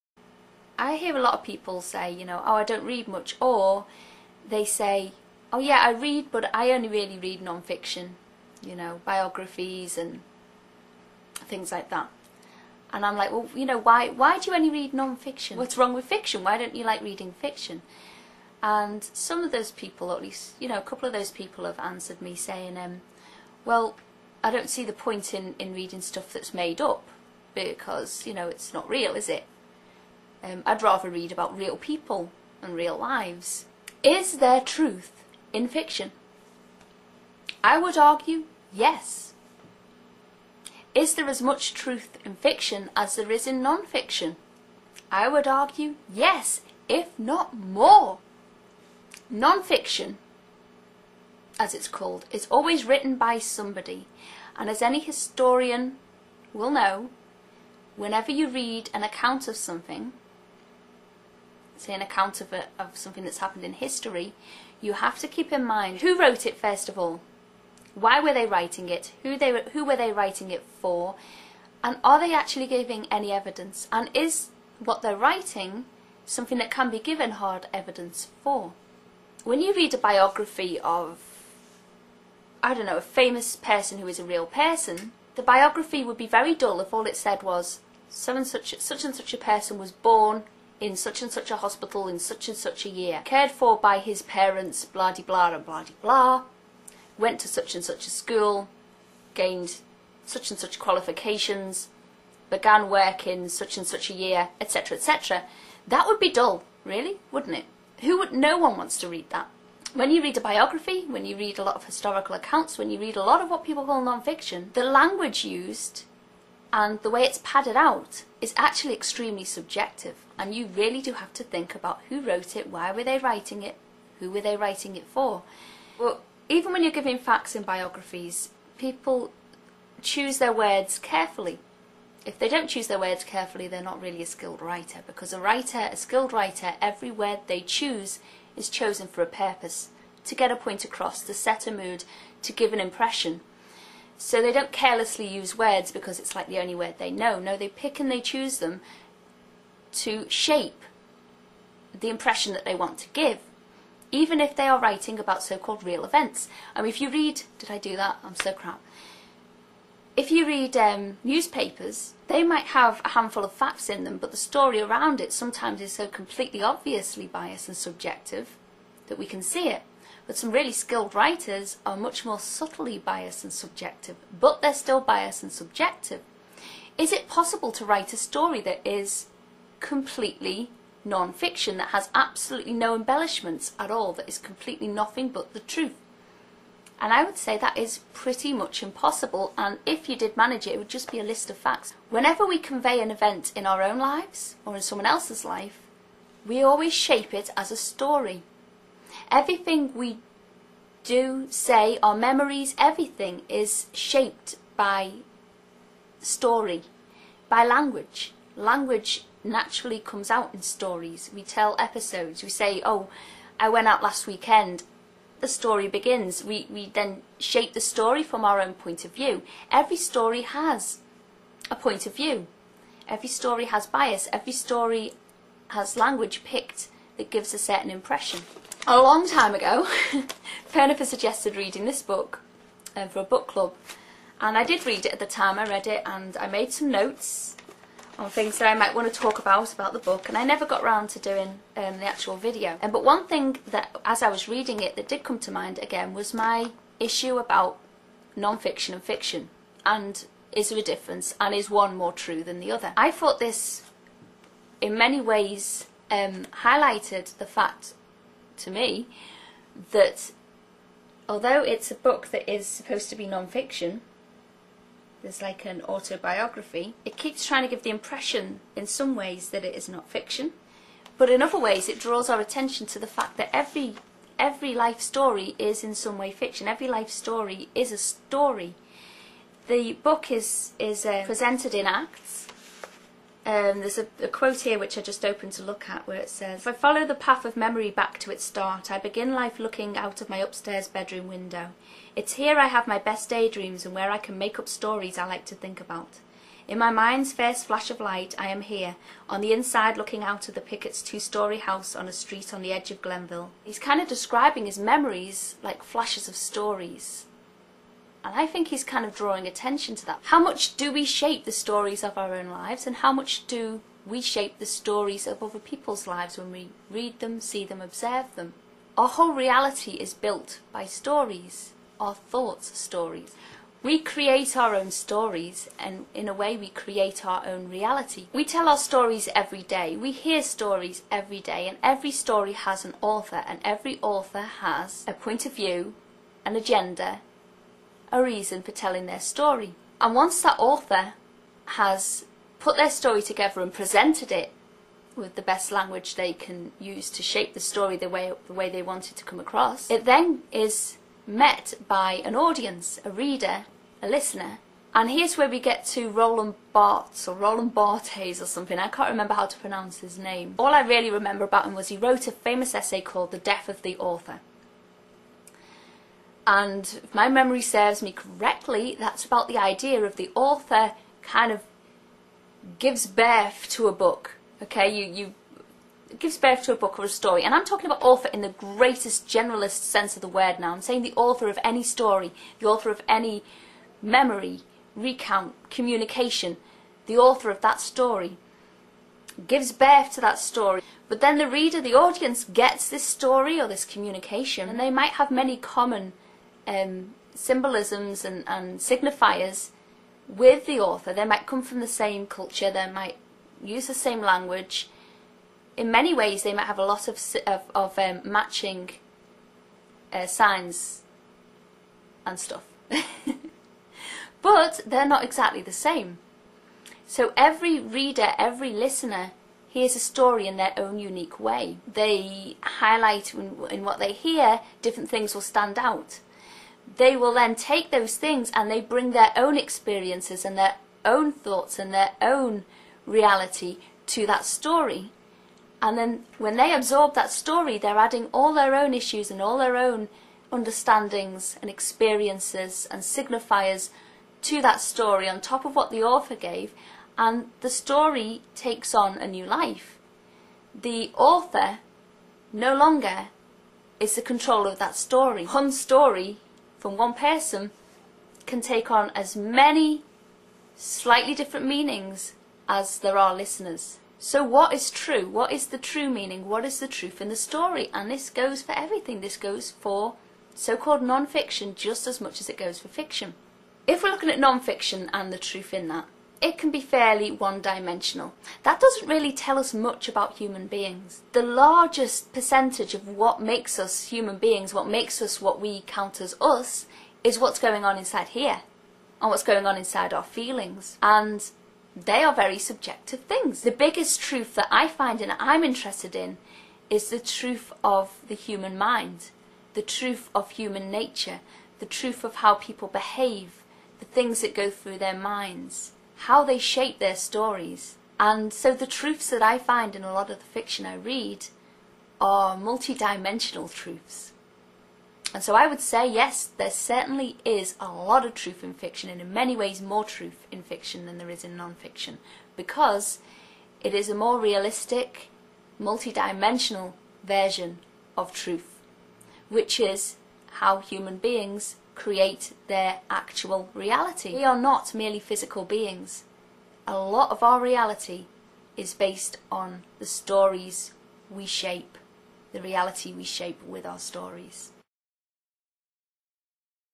Reality In Fiction?
This is a subject I feel very passionately about, and I have been wanting to do a video on it for quite some time! It's not really something that can be fit neatly into one video, I could discuss and give examples for hours, but I think 9 minutes is more than enough for most YouTube viewers! Maybe some of you will find it interesting and join in the discussion!

The book I refer to was recommended for a 'book club' by furnifer. It's a good book, apparently there is a film too, but I haven't seen that!
Finding Fish by Antwone Fisher.

Roland Barthes wrote the essay 'The Death of the Author', I'm sure that's available online somewhere if you want to read it, it's one of the first things we looked at in my English Literature degree course. I don't really address the points the essay makes, but it's still relevant!